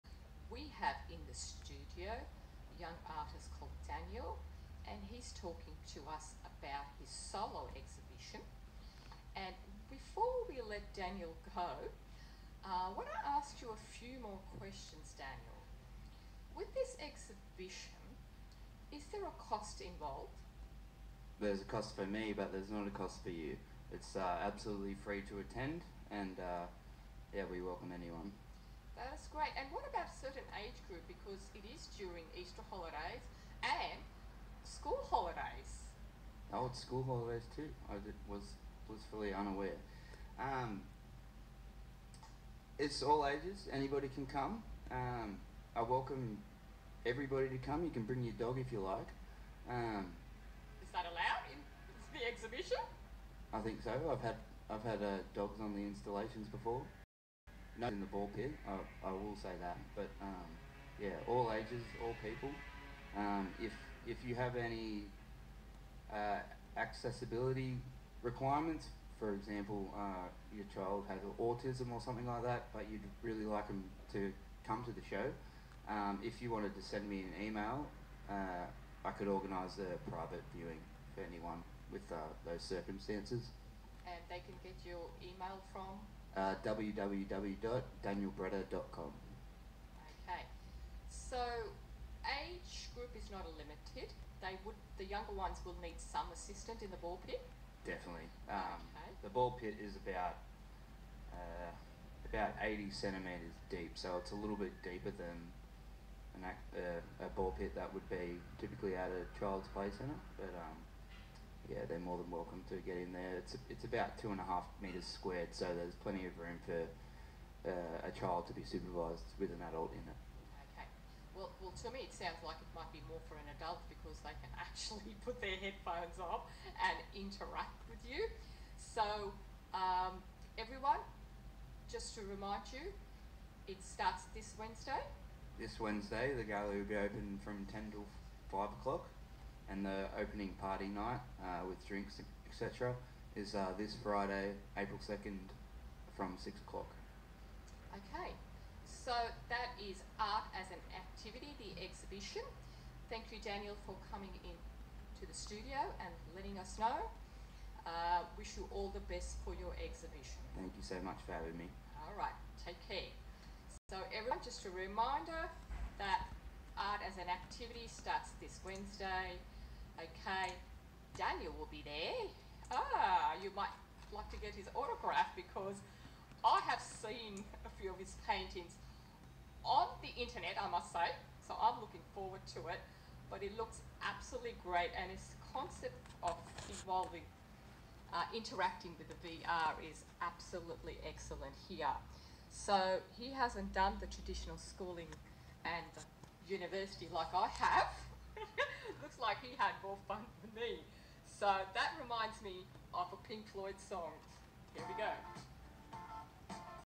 Art As Activity - VoxFM -Virtual Reality Ball Pit Interactive Art - Daniel Breda
'Art As Activity' explores the concept of ‘dynamic sensation’ in neo-futurist art through the synchronization of virtual reality and installation art. The exhibition will portray Breda’s love of creating active participation in the gallery space, alongside his painting practice to form juxtapositions between innovative technology and traditional art.

Described by the Italian Futurists in 1909, ‘dynamic sensation’ involved artists representing their ‘whole environment’. This included depictions of machines, nature, humans and the movements of such; presented in unification. In contemporary space, our environment also includes the digitalised spaces of screen culture, digital interfaces and virtual environments. To convey this concept in a neo-futurist manner, this project will create active participation through the synchronization of the Virtual interface with real-life experience through installation and virtual reality art.

Executed with the Oculus Quest 2, Unity 3d, and a physical installation in collaboration with Virtual Reality developer Brennan Hatton, ‘Art as Activity’ will personify this concept in the form of a physical and virtual ball pit. Traditionally a play environment, the ball pit assists children to develop their sensory motor skills in their formative years. Similarly, VR has been used in contemporary space as an extension of human cognition. The synchronization of these tools will form an uncanny unification of real and virtual space as we know it. Enabling gallery participants to be immersed in a physical ball pit, whilst interacting with the VR ball pit. The simplicity of the ball pit will identify the contemporary human relationship with our ‘whole’ environment and understanding of real space.

‘Art As Activity’ will be on view at Project Contemporary ArtSpace Wollongong from March 31st - April 11th

Opening night Friday April 2nd, 6pm.

Presented by Daniel Breda.

Supported by the City of Wollongong